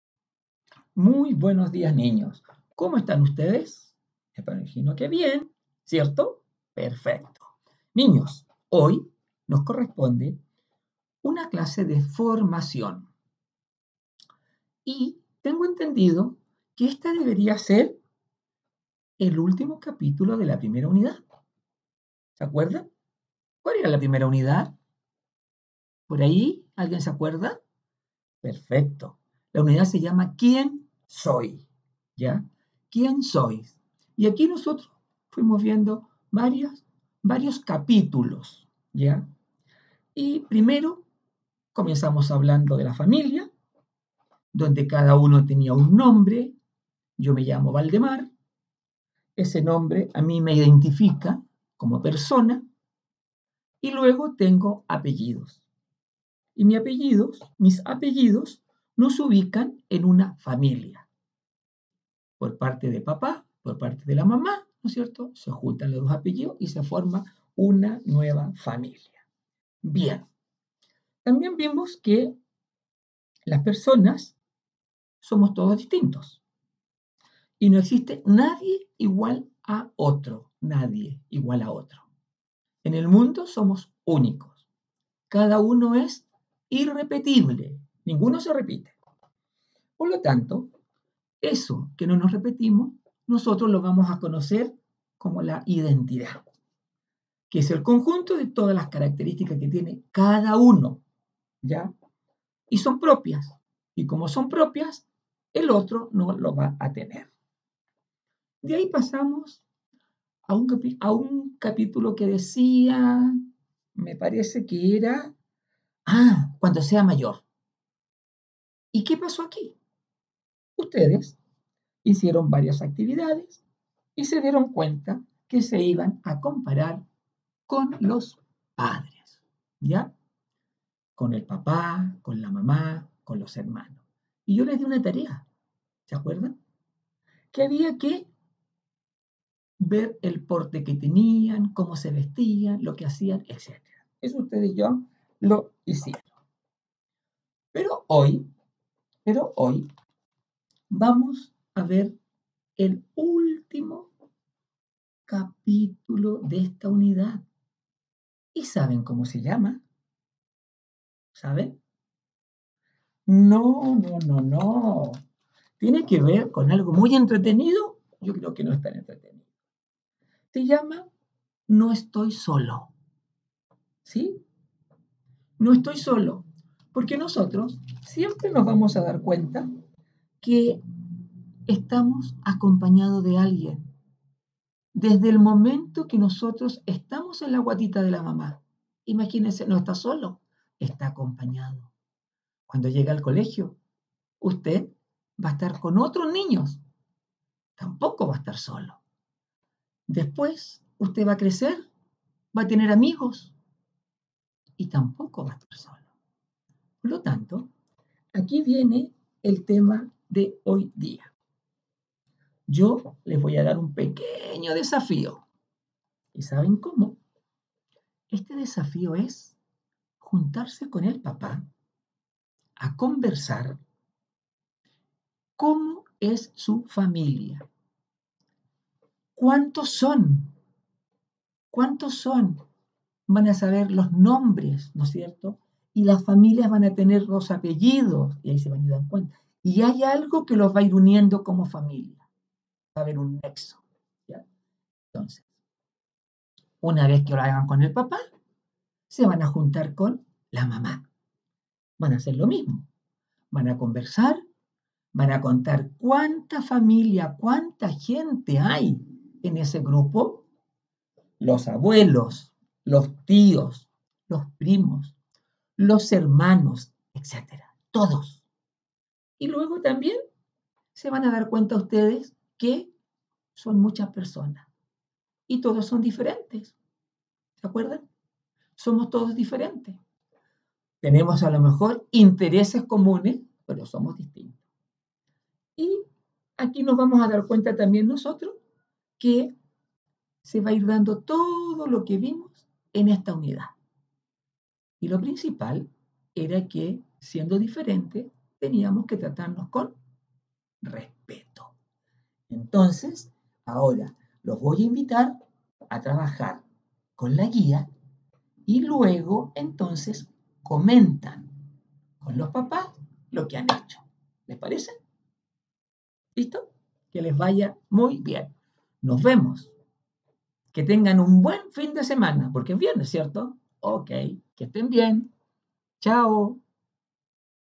Formación: "Nuestra identidad, somos únicos". Primero Básico. Colegio Tabancura
Cuarta clase de Formación para Primero Básico correspondiente al viernes 29 de mayo. En esta clase se trabajará la identidad personal y la importancia de aceptarnos tal cuál somos.